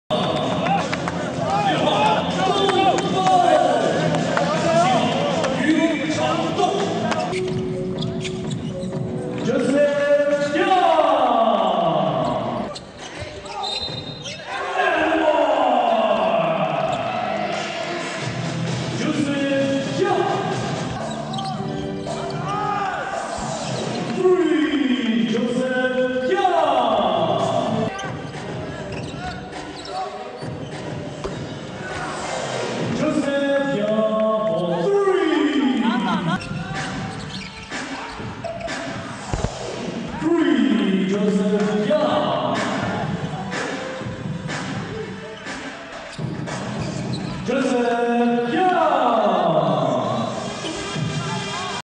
Joe Young scores game-high 27 points in 23 minutes in his debut for CBA Beikong Royal Fighters.
Welcome back to CBA! 约瑟夫·杨北控首秀集锦
Former Pacers player Joe Young scores game-high 27 points in 23 minutes in his debut for Stephon Marbury's Beikong Royal Fighters.